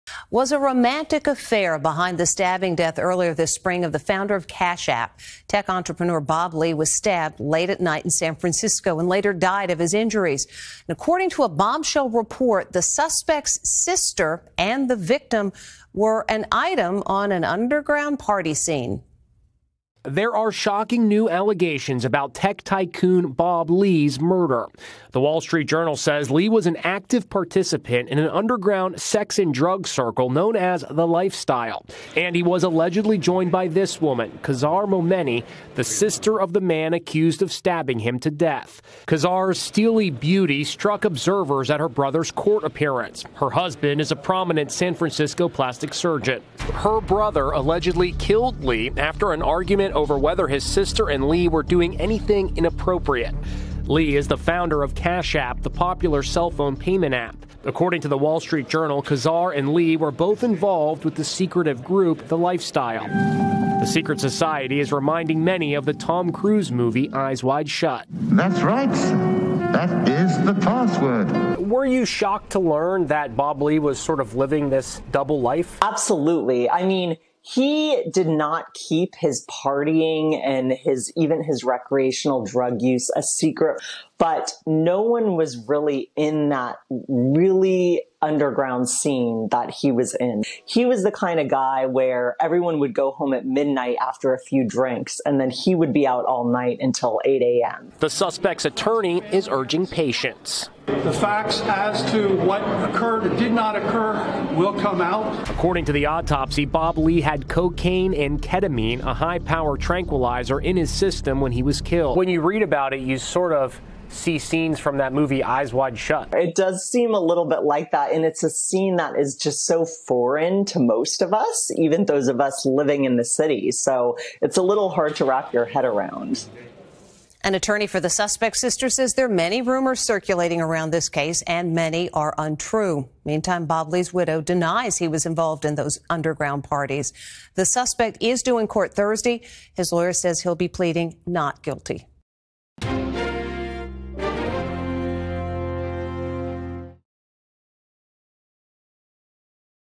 Cash App Founder Visited Underground Sex/Drug Parties With Alleged Killer's Sister
The Wall Street Journal says Lee was an active participant in an underground sex and drugs group known as "The Lifestyle" and that he was allegedly joined by the sister of the man accused of stabbing him to death.